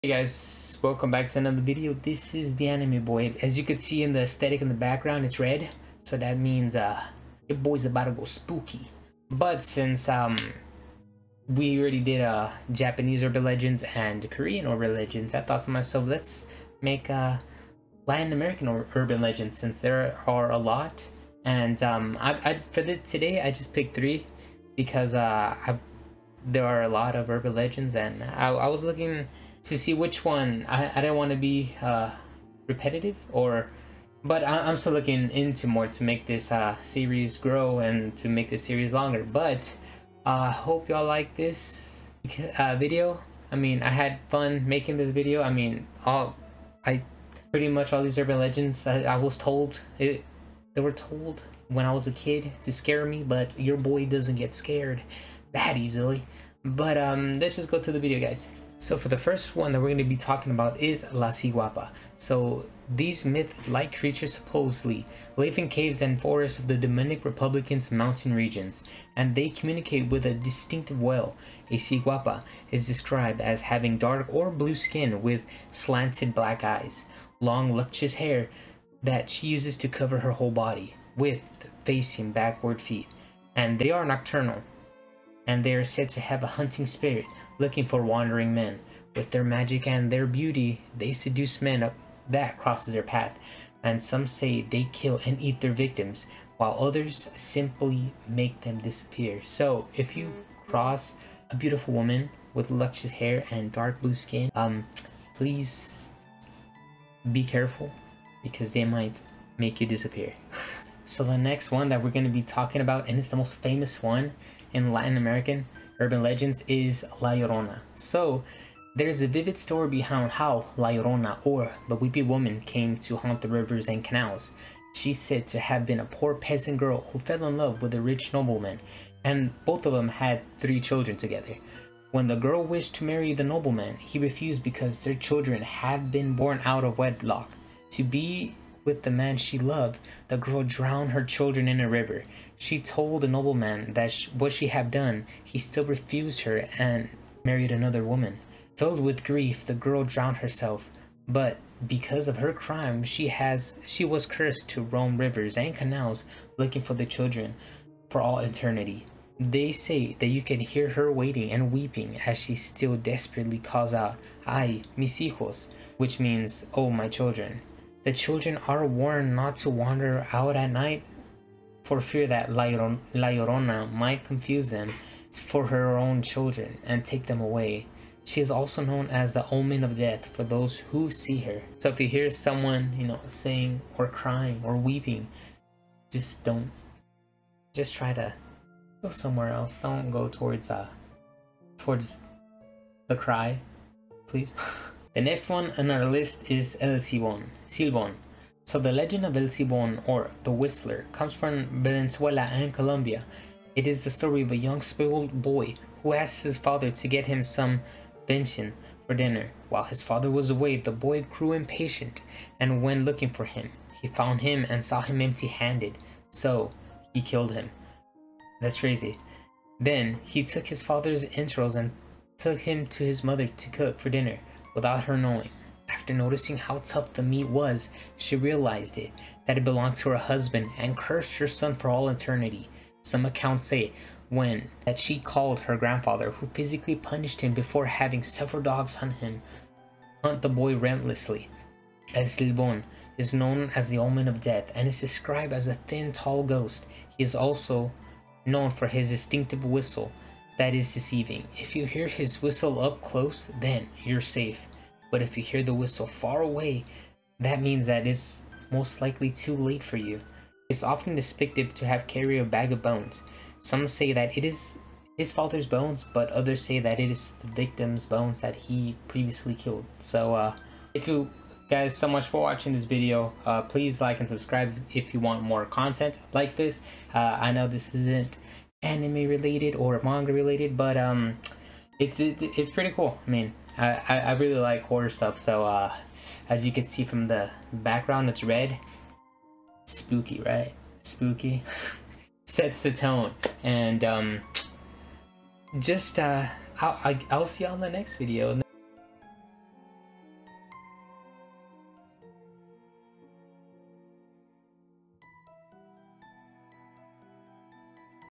3 Latin Urban Legends THAT YOU SHOULD KNOW!! !!
this is the anime boy and these are 3 Latin urban legends that i remember when i was a boy and well i wanted to share these with you